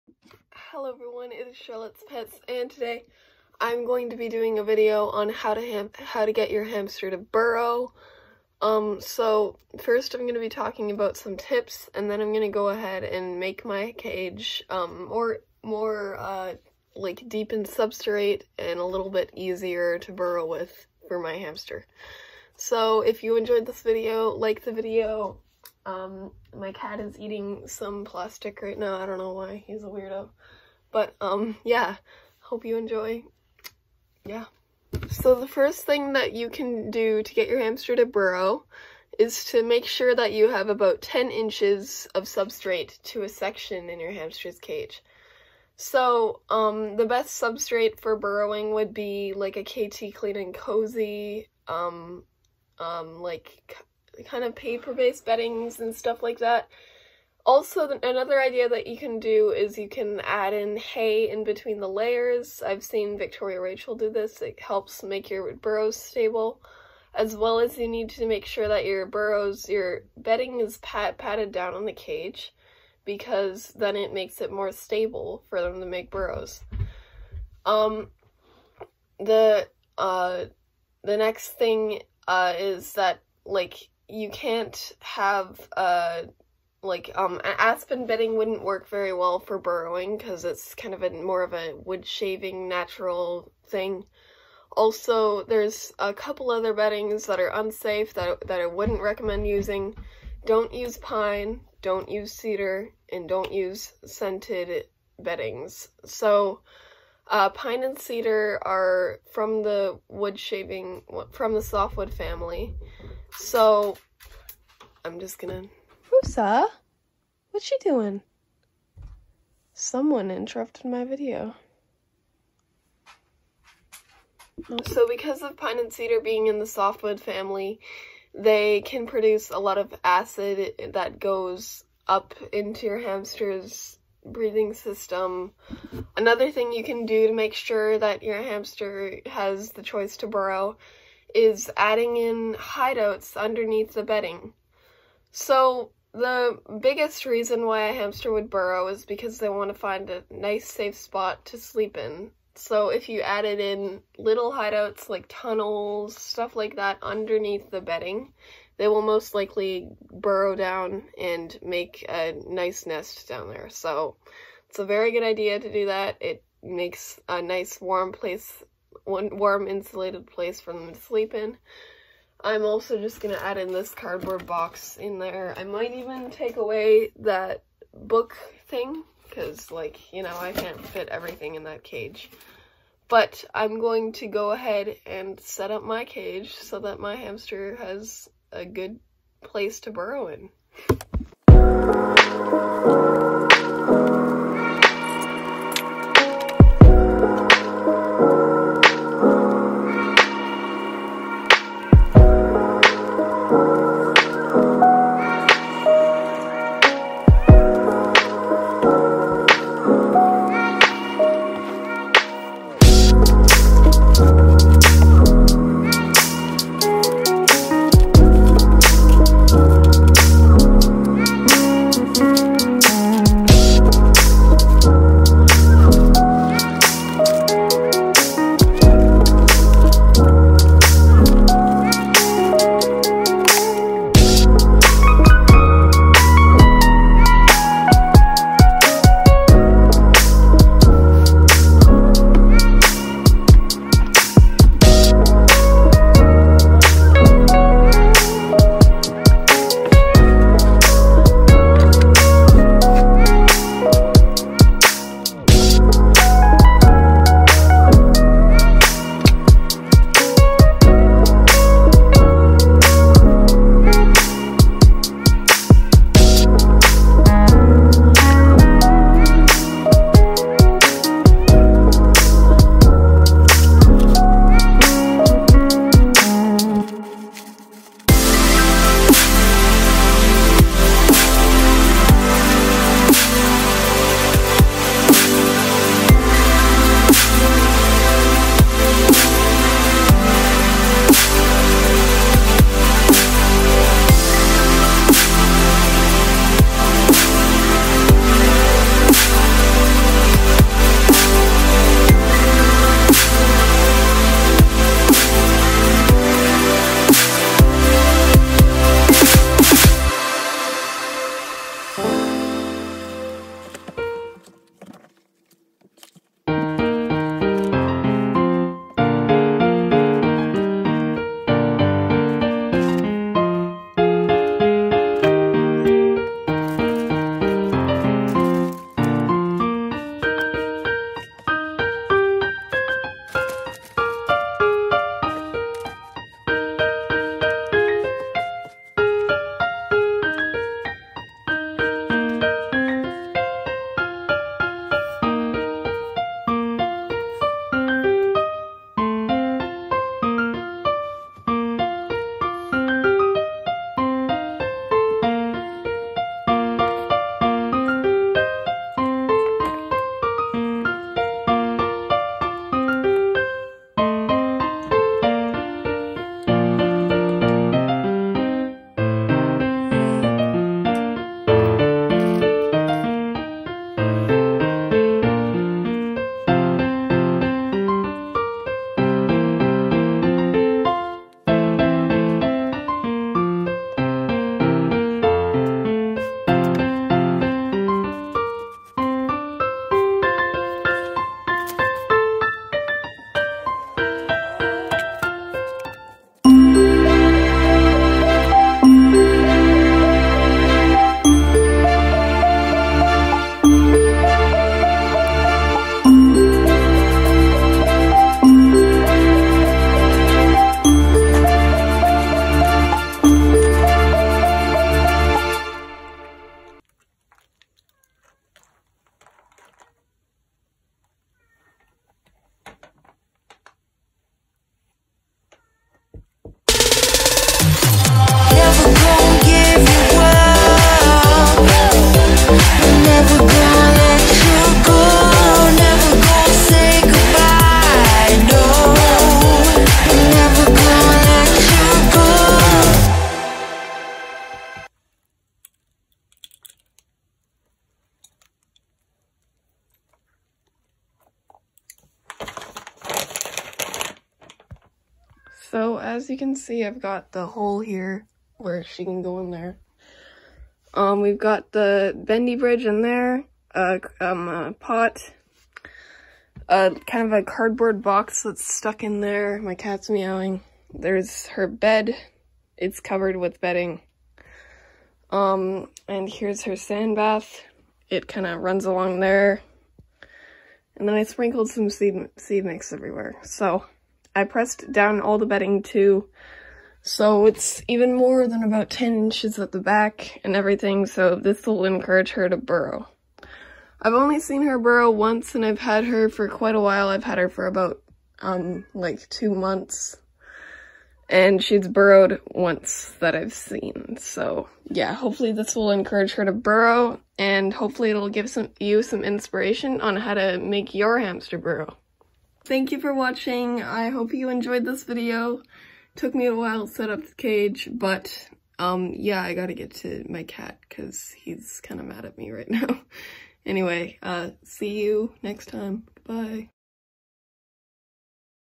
Getting your Hamster to BURROW! |Encouraging natural behaviours
I really hope this video gave you guys some inspiration, and I think it’s great to encourage natural behaviours. In this video, I included some tips, and representation of how you can do this with your hamster cage.